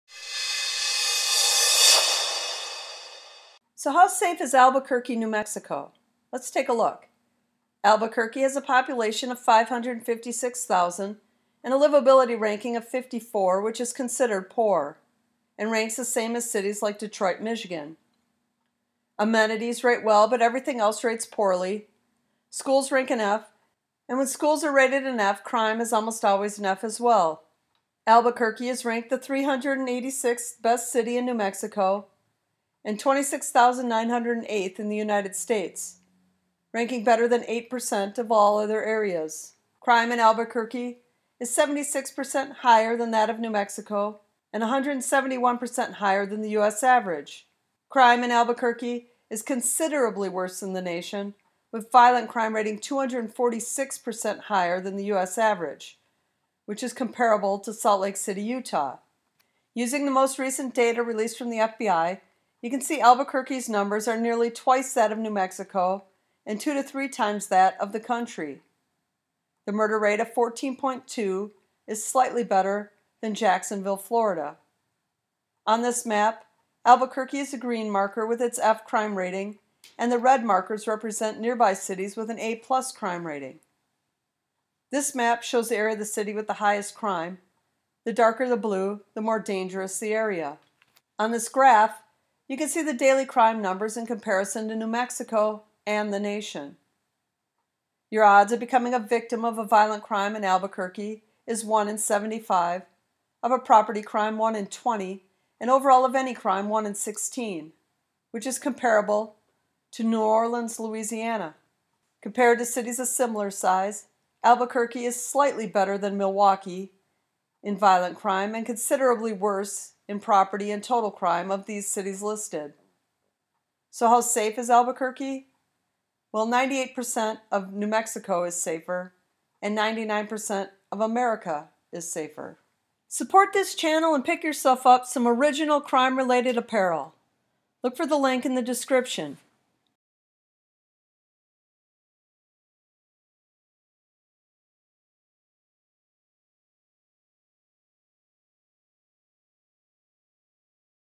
Is Albuquerque NM one of America's Most Dangerous Cities? How Safe is Albuquerque?
Ever wonder how safe your city is? We take a look at the statistics and how it ranks among other cities in its state as well as how it compares across America.
Violent Crime Statistics
Property Crime
Murder rates
Overall Crime, including your chances of becoming a victim.
How Livable the city is. Safer nearby cities. How it compares to other US cities.